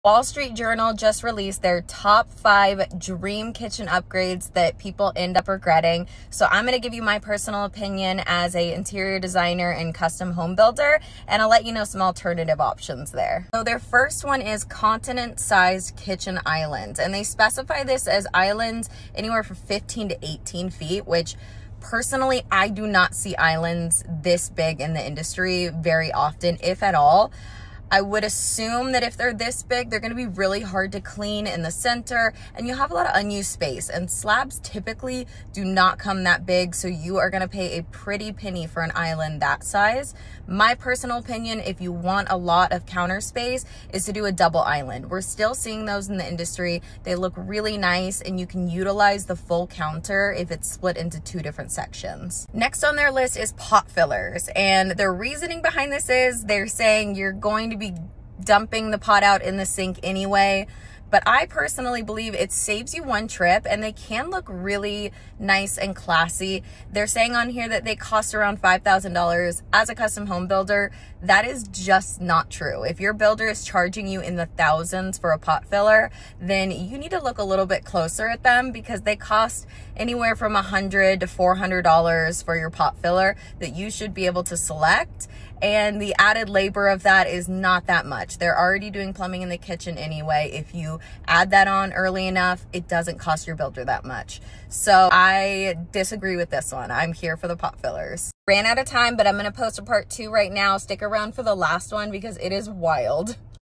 Top 5 dream kitchen upgrade “regrets”, part 1 of 2
My review of the just released top 5 dream kitchen upgrade regrets, as a custom home builder and designer. 🖤